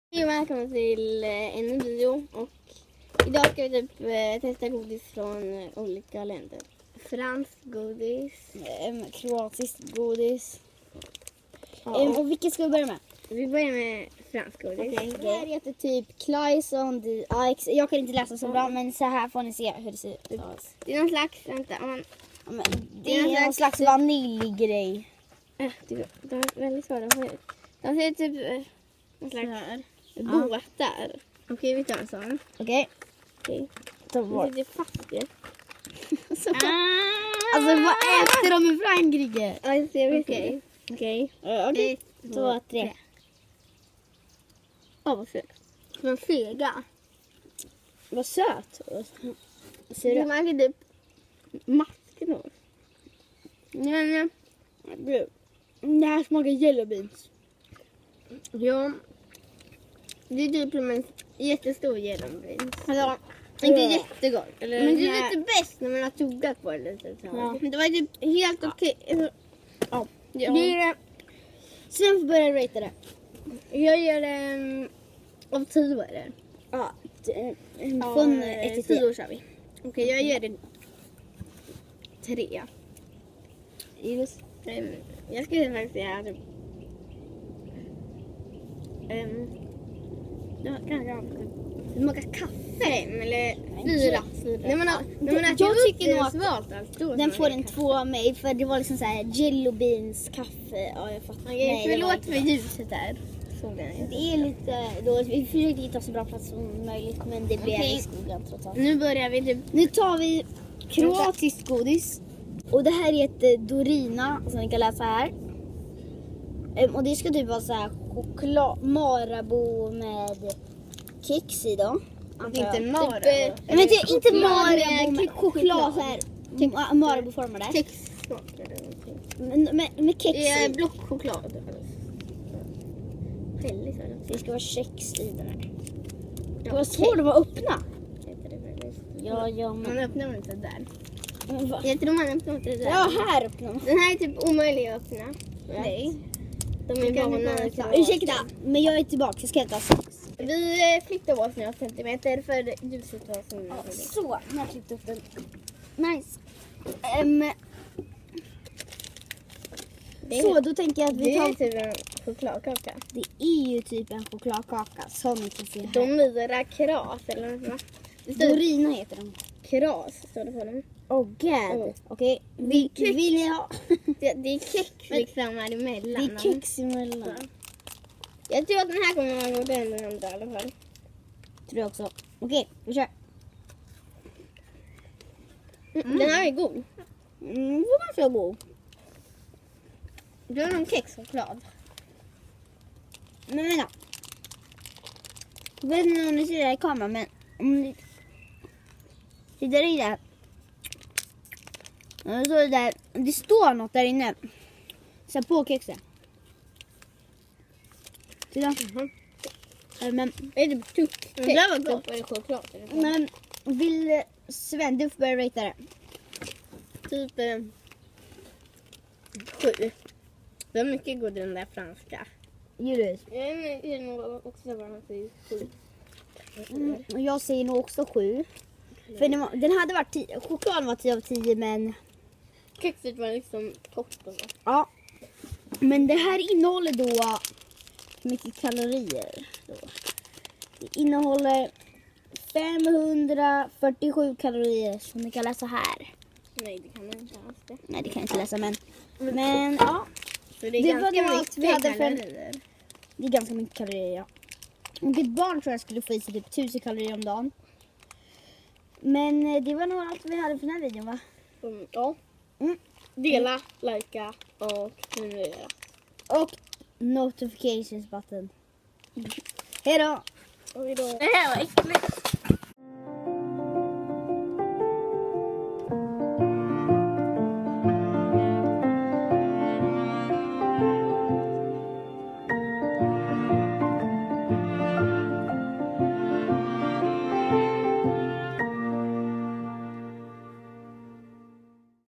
Testar Godis Från Kroatien & Frankrike
Kroatiskt godis=gott. Franskt godis=äckligt.